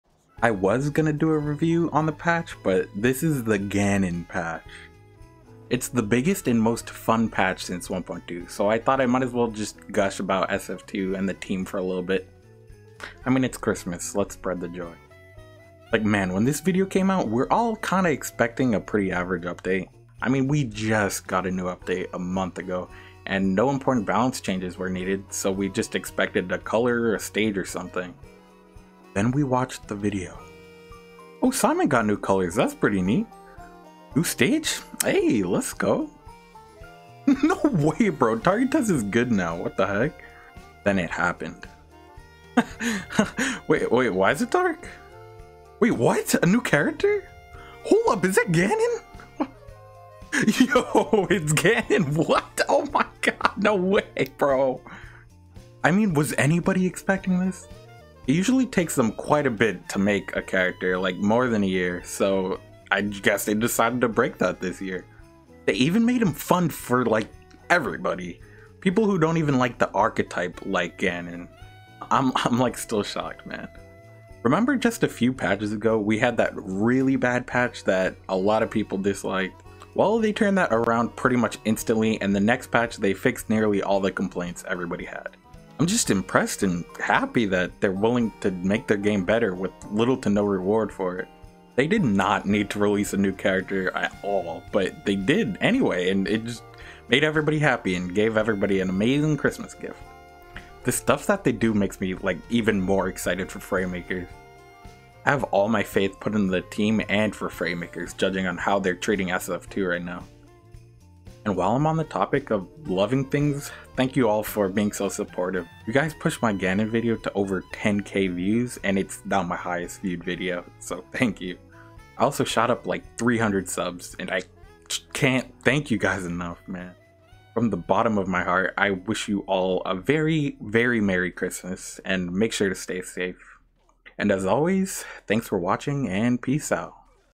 Gushing over SSF2 1.3 and McLeodGaming
Have a great Christmas guys! Im super looking forward to the future of this community. I know its going to be great :D